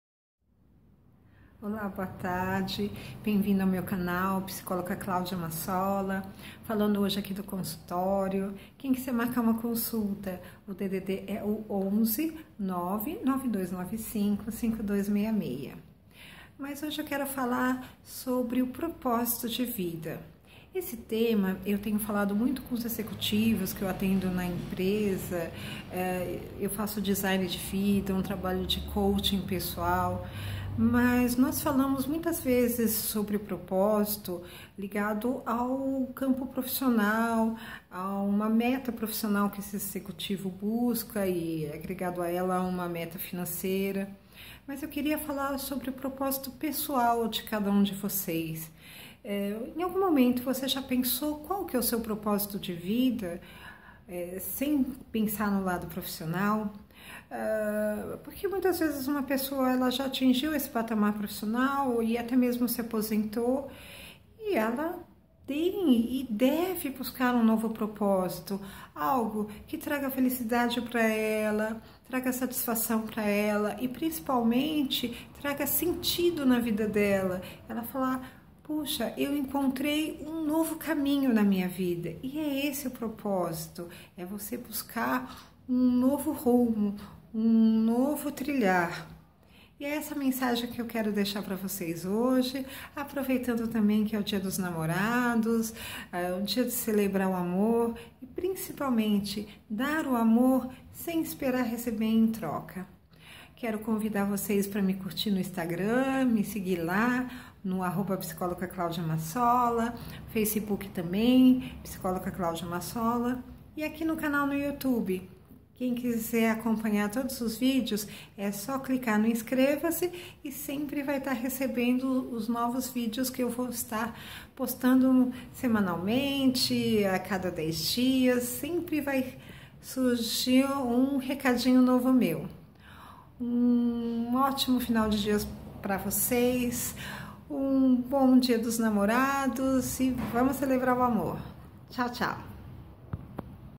A Psicologia e o Propósito de Vida
Nesse vídeo falo não apenas do propósito profissional mas,  principalmente, do propósito pessoal de cada um de nós.